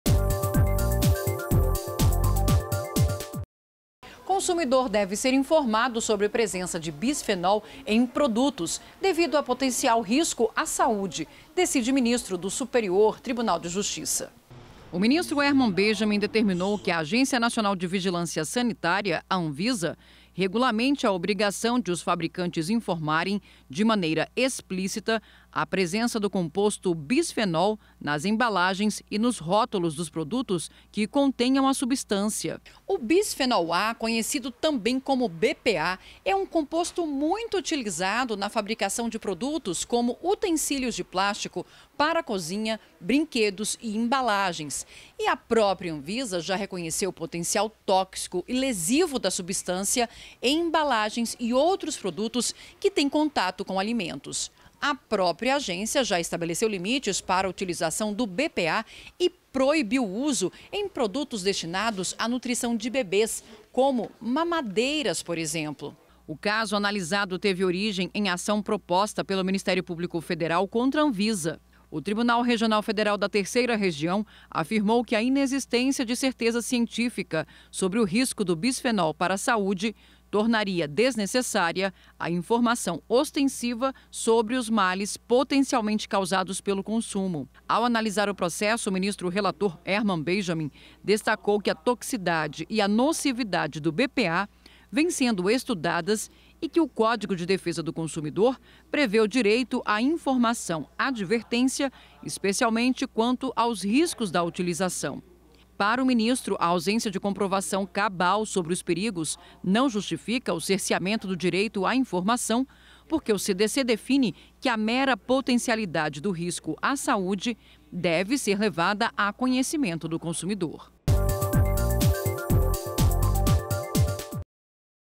Potencial risco à saúde exige que consumidor seja informado sobre presença de bisfenol em produtos
​O ministro Herman Benjamin, do Superior Tribunal de Justiça (STJ), determinou que a Agência Nacional de Vigilância Sanitária (Anvisa) regulamente a obrigação de os fabricantes informarem, de maneira explícita, sobre a presença do composto bisfenol A (BPA) nas embalagens e nos rótulos dos produtos que contenham a substância.
O bisfenol A é um composto muito utilizado na fabricação de produtos como utensílios de plástico para cozinha, brinquedos e embalagens. Segundo o ministro, a própria Anvisa já reconheceu o potencial tóxico e lesivo da substância em embalagens e outros objetos que têm contato com alimentos, tanto que estabeleceu limites para sua utilização e a proibiu em produtos destinados à nutrição de bebês (como mamadeiras).